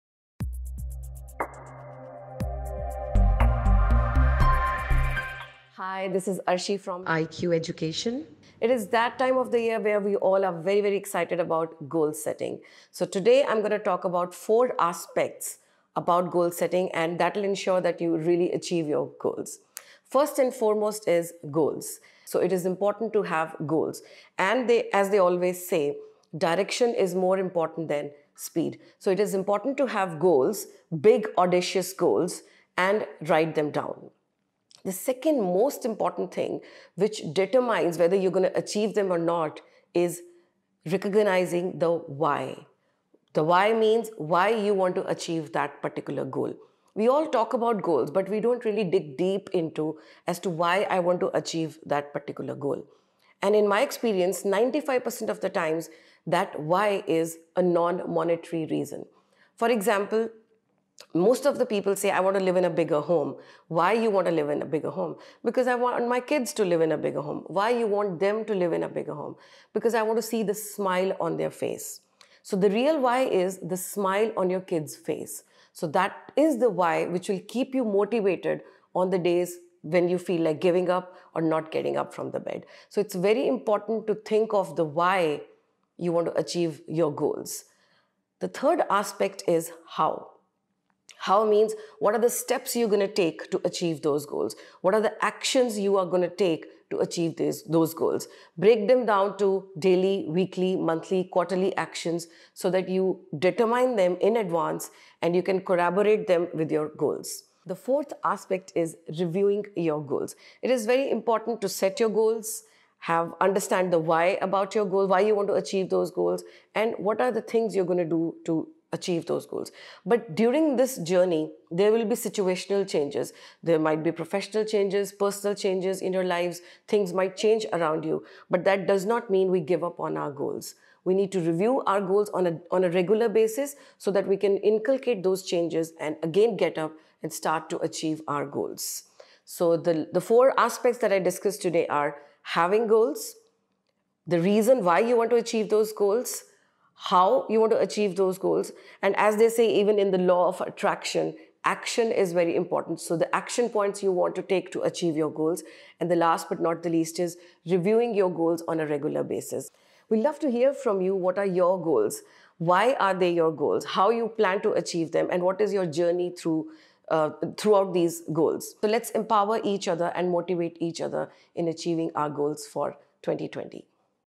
4 Basic aspects of goal setting by CA. Arshi Kaura | New Year Goals
Have you set your goals for the New Year and the New Decade ? What are the 4 aspects of Goal Setting ? Know from CA. Arshi Kaura – an important cog of the team IQ.